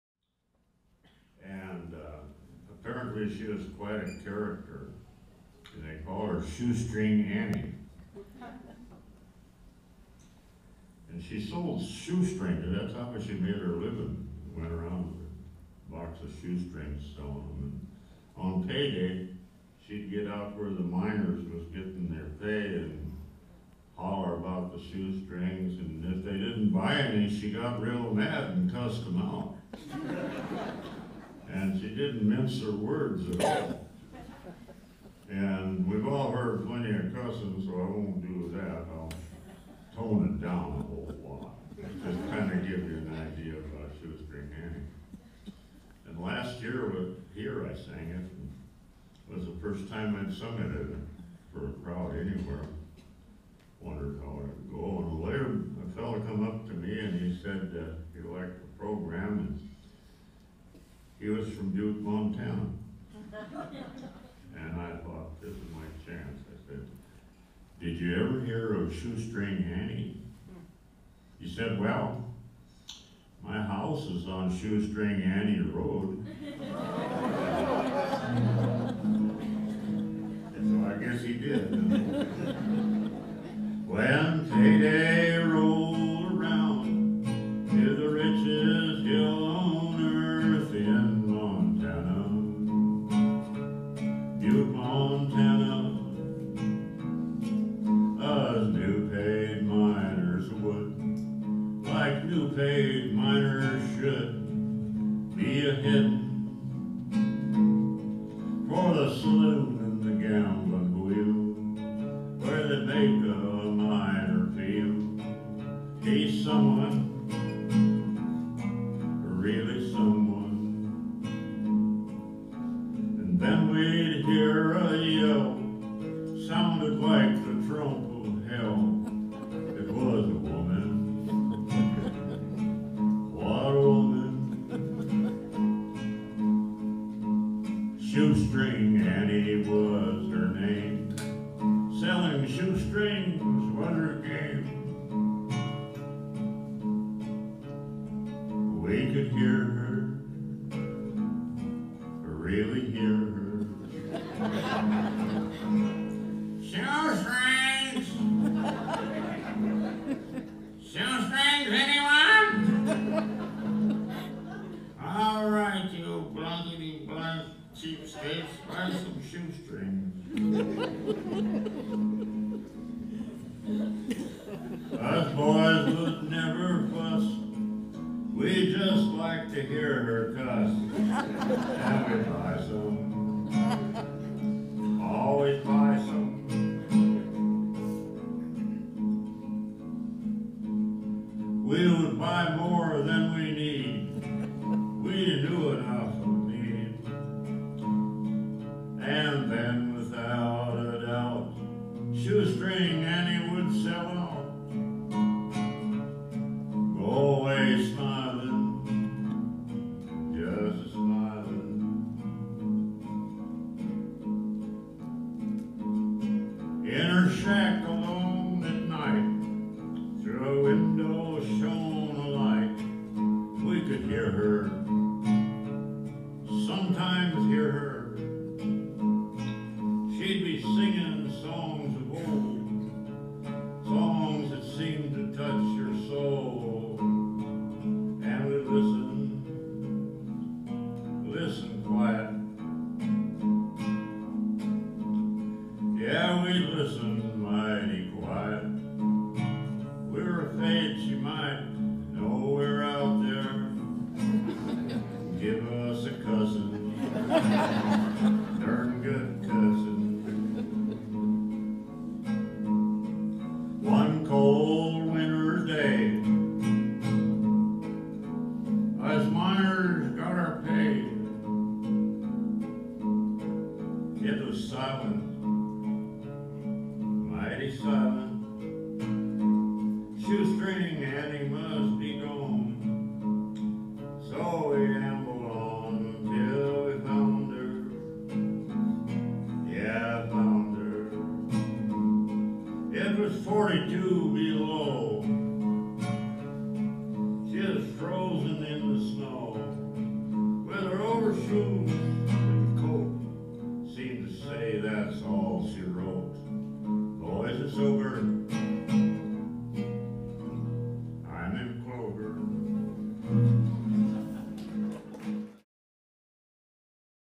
GlennOhrlin
Glenn Ohrlin telling a story about Shoestring Annie--legendary character from Butte, Montana.  This video is from the 2013 National Cowboy Poetry Gathering in Elko, Nevada.  More about the gathering can be found at The Western Folklife Center